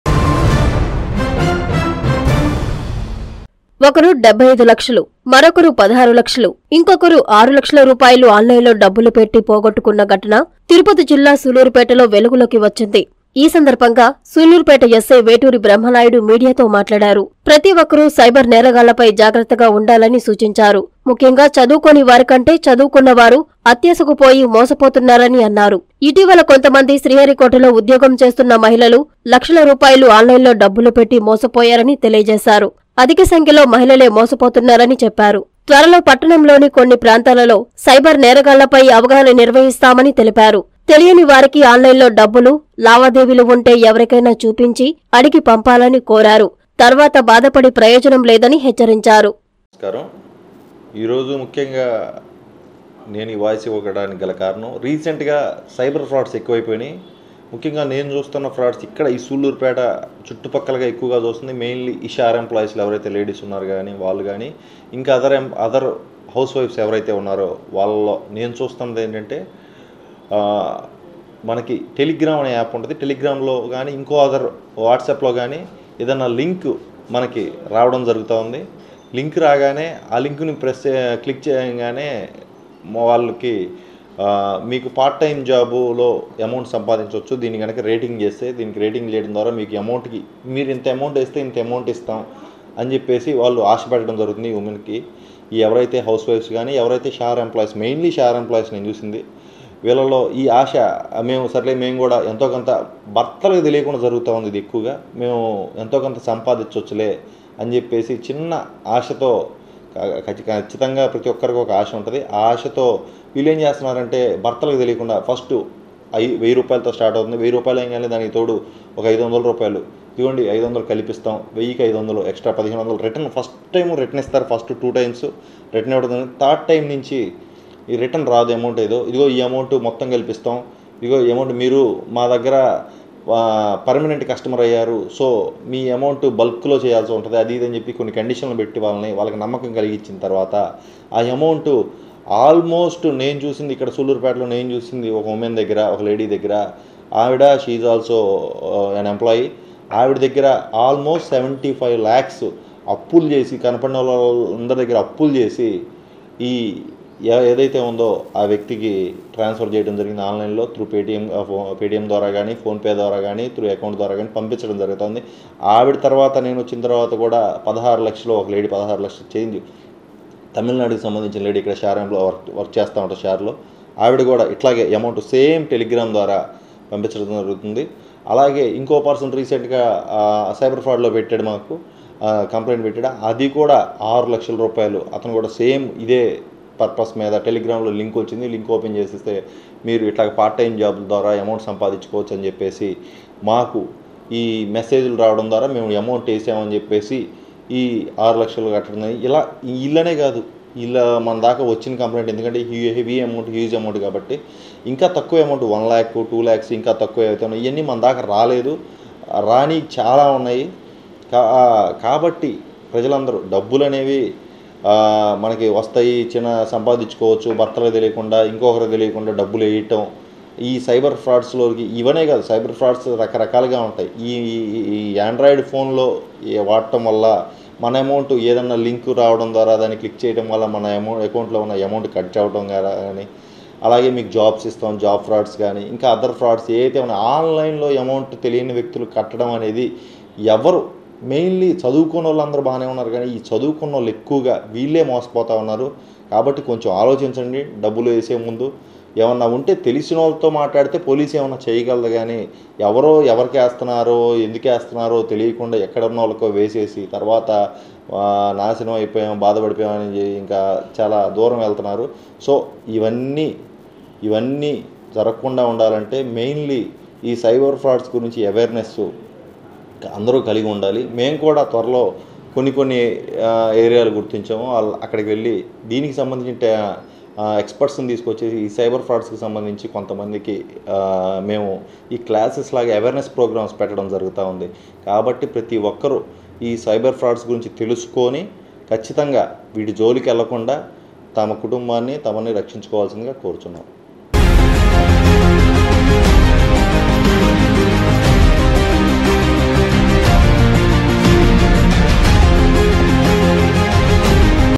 ల‌క్ష‌లు పోగొట్టుకున్న మ‌హిళ‌లు | Cyber Crime in Sullurupeta | SI Brahmanaidu
- టెలిగ్రామ్, వాట్సాప్ లింక్ పేరుతో..సైబర్ మోసం..!
- సూళ్లూరుపేట‌లో వెలుగులోకి వ‌చ్చిన ఘ‌ట‌న
- సైబర్ నేరాలపై జాగ్రత్తగా ఉండాలి - ఎస్సై వేటూరి బ్రహ్మనాయుడు..